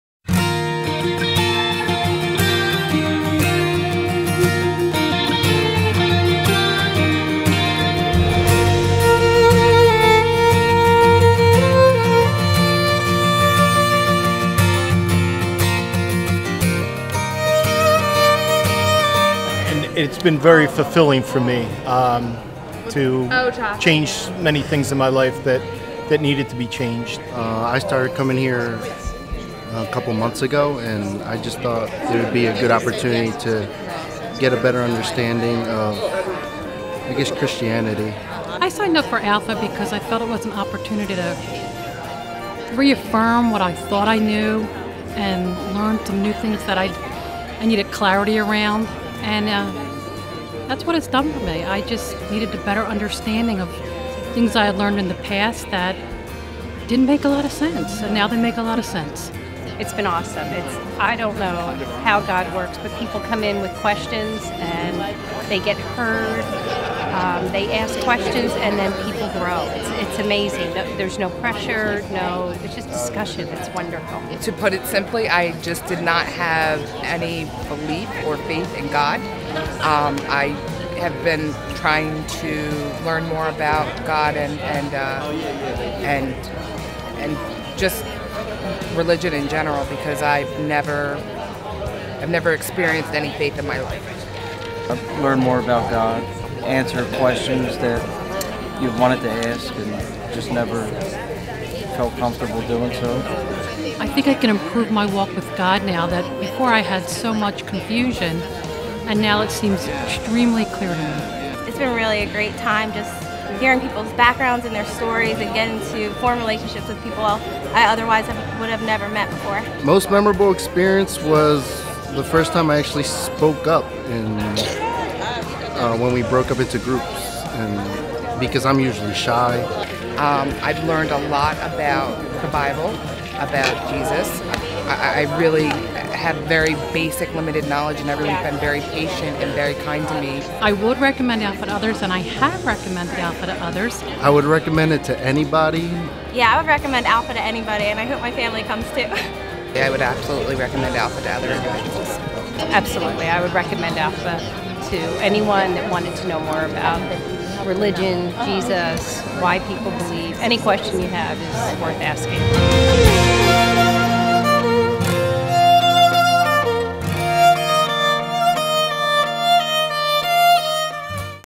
Alpha Promo for January 2014 Alpha at Sovereign Grace Church
Alpha Promo for January 2014 Alpha at Sovereign Grace Church, 111 Greentree Road, Marlton, NJ